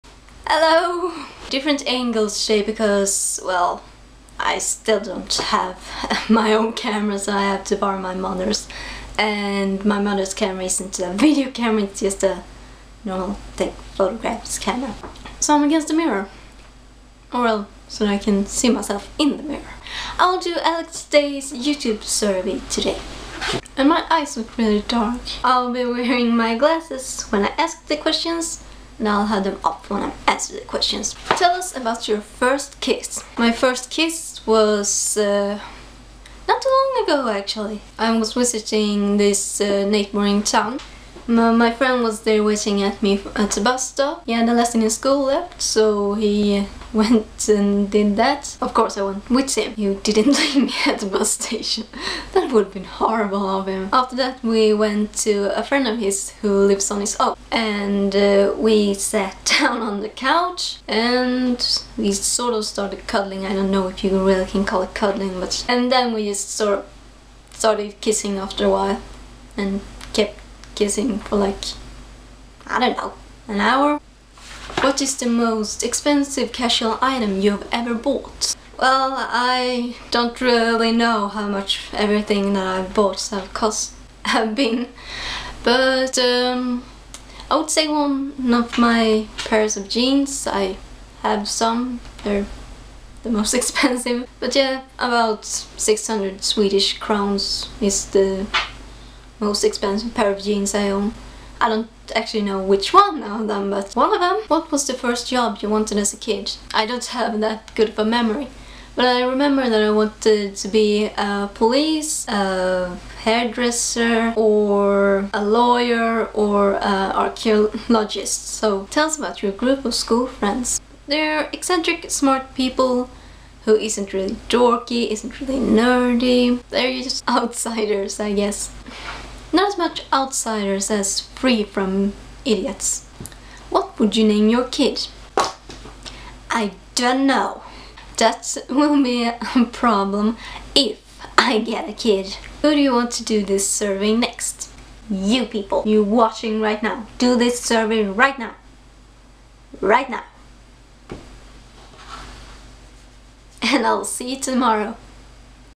And on the second week...
...there were darkness. And some answers to Alex Day's survey, I'll be looking forward to your answers. :3